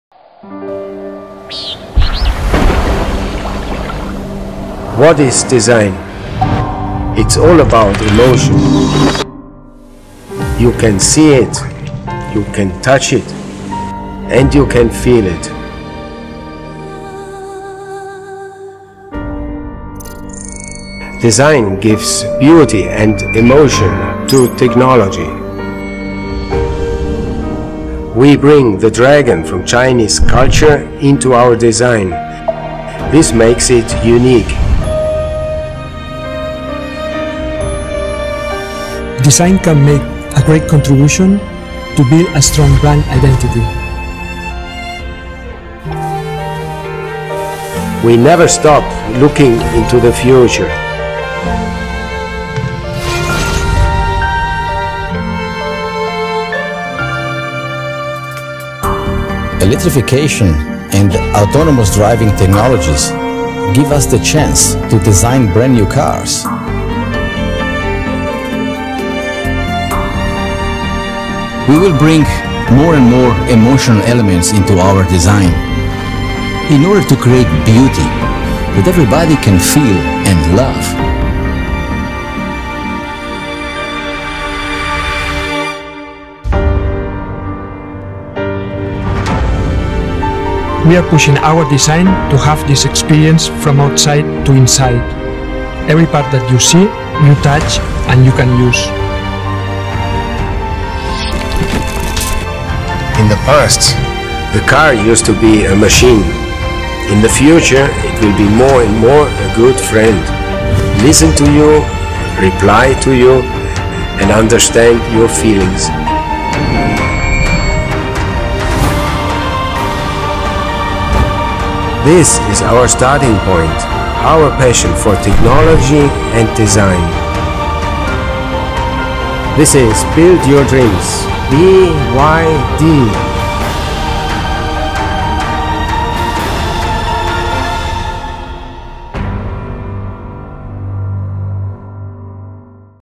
The BYD Design Philosophy
“Technology is BYD's hard strength, and design will become the soft strength of the company.”
Wang Chuanfu, President and Chairman of BYD.

Wolfgang Egger - BYD Global Design Director
JuanMa López - BYD Global Exterior Design Director
Michele Jauch-Paganetti - BYD Global Interior Design Director